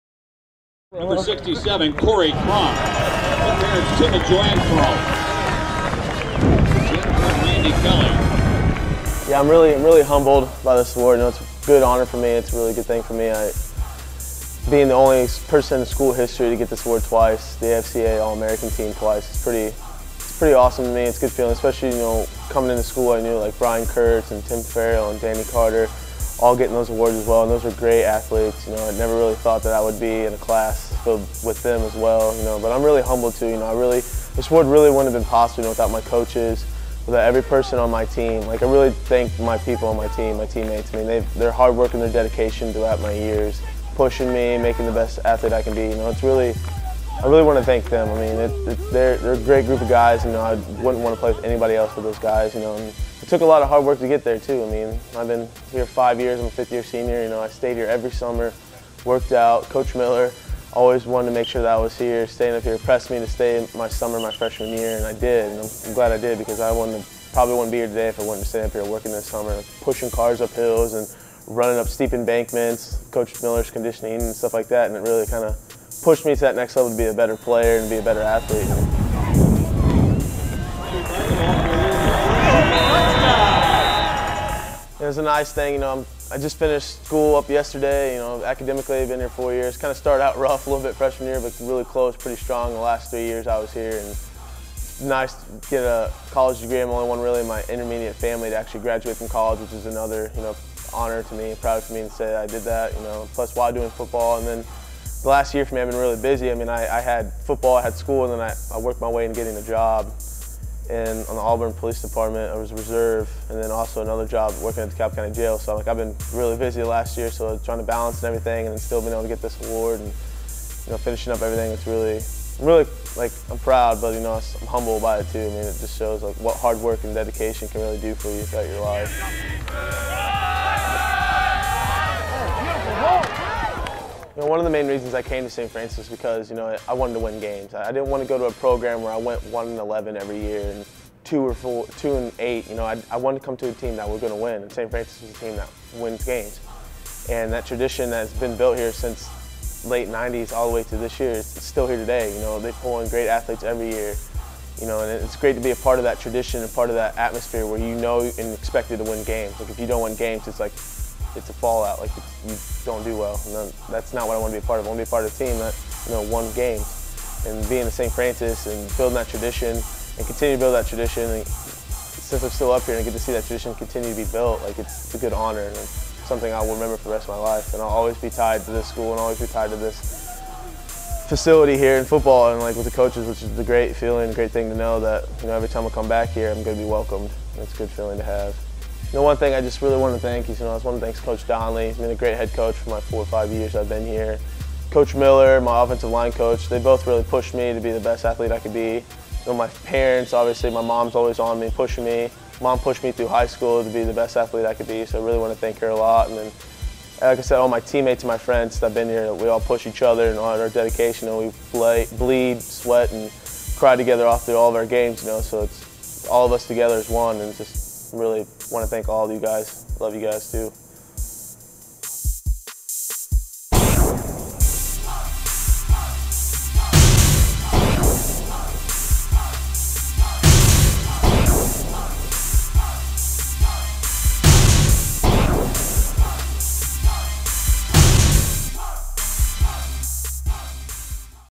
2011 - Football Corey Cronk AFCA ALL-AMERICAN
University of Saint Francis senior offensive guard Corey Cronk has been named to the American Football Coaches Association NAIA All-America Team for a second consecutive year. This is the fifth time in the last six years USF has had a player selected for the AFCA NAIA All-American Team.
   Cronk (6-foot-1, 315, Fisher, IN/Hamilton Southeastern H.S.) becomes the first member of a Cougar squad to earn the prestigious award twice. He is one of three player named from the Mid-States Football Association Mideast League, the only one on the offensive side of the ball. Cronk is a two-time All-MSFA MEL first-team selection.
  He has started in 33 games over the last three seasons on the Cougars' offensive line. Corey is the son of Tim Cronk and Jennifer Kelly. He is a sociology / criminal enforcement major at USF.